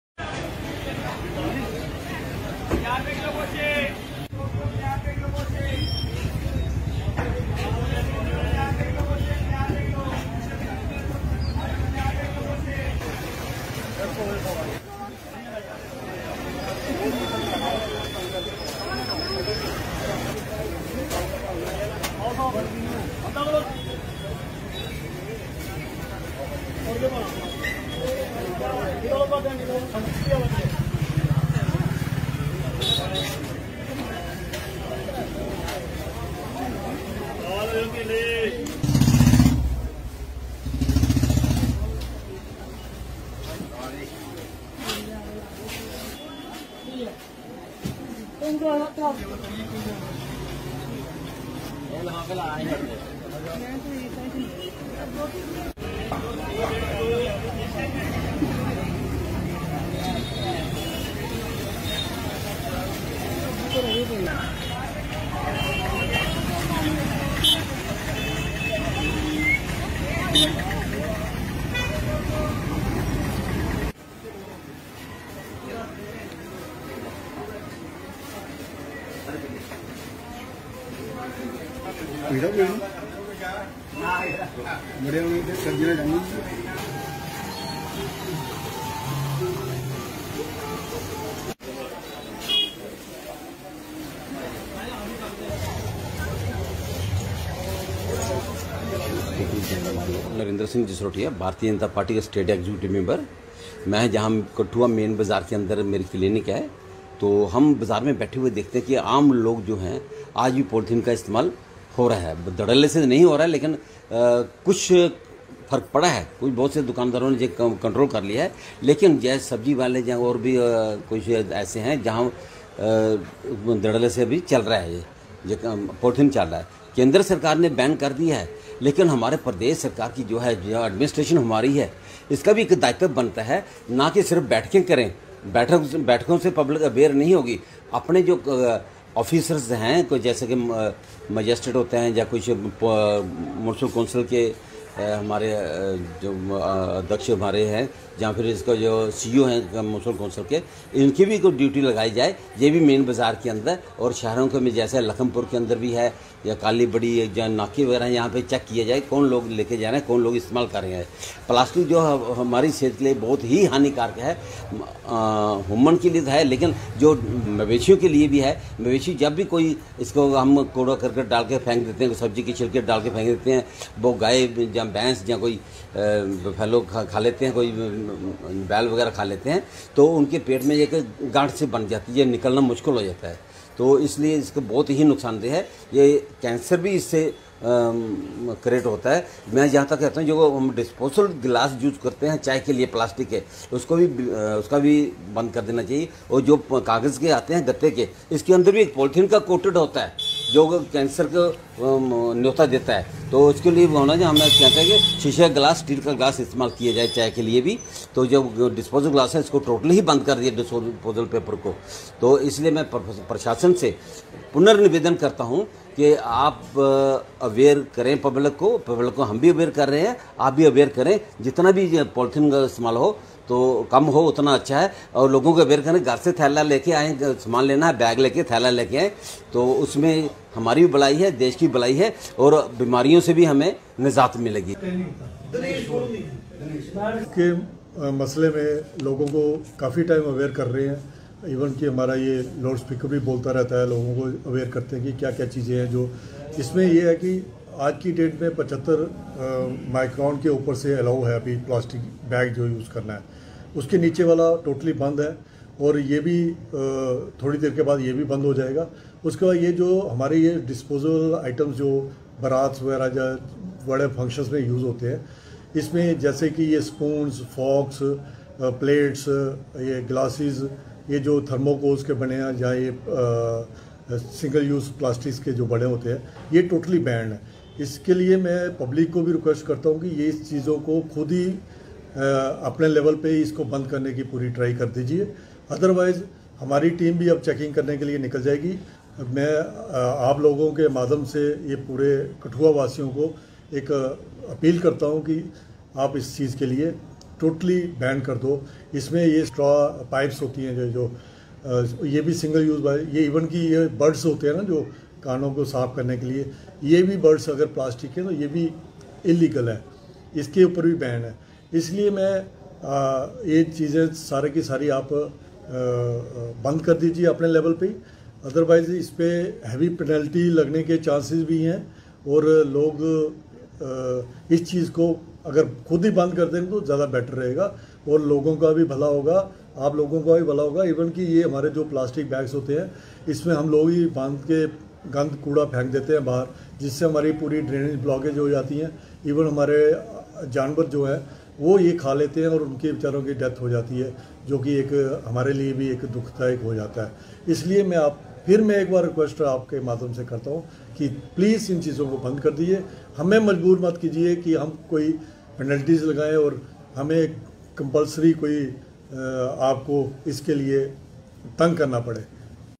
केंद्र सरकार द्वारा प्रतिबंध के बाद भी कठुआ शहर में खुलेआम बिक रही पॉलीथीन
केंद्र सरकार द्वारा प्रतिबंध के बाद भी कठुआ शहर में खुलेआम बिक रही पॉलीथीन, जिला प्रशासन द्वारा नहीं रखी जा रही कोई नजर , भाजपा नेता ने प्रशासन को बताया लापरवाह